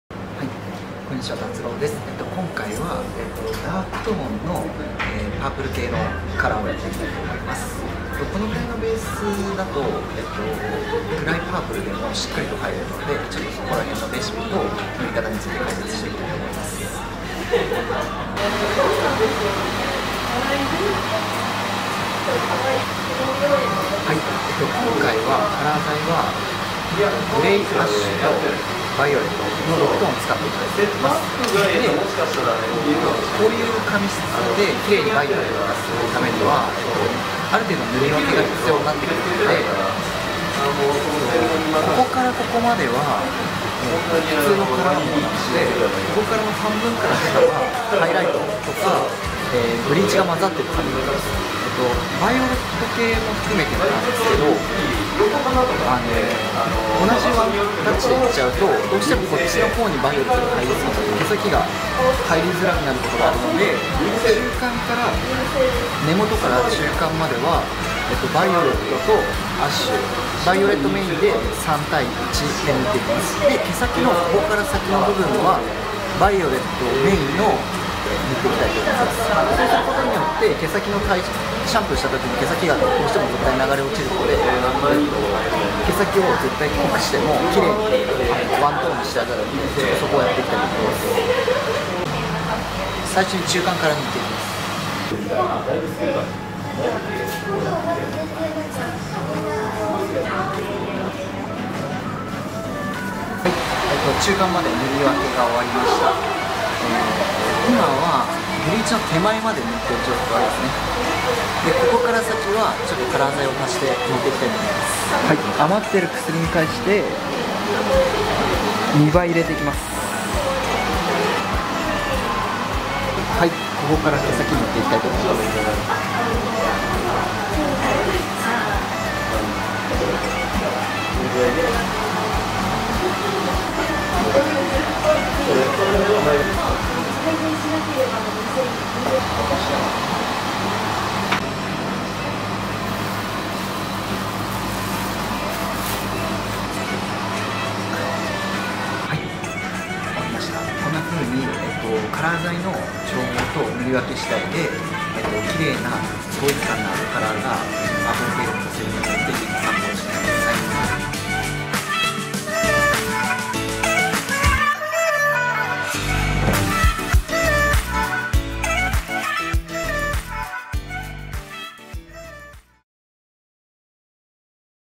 【基本テクニック】パープル・バイオレット系のカラーを綺麗に出すコツ
パープル・バイオレット系のカラーをムラなく出す基本テクニックです。
それぞれいろんな考え方やレシピがあるので、少しでも参考になれば幸いです！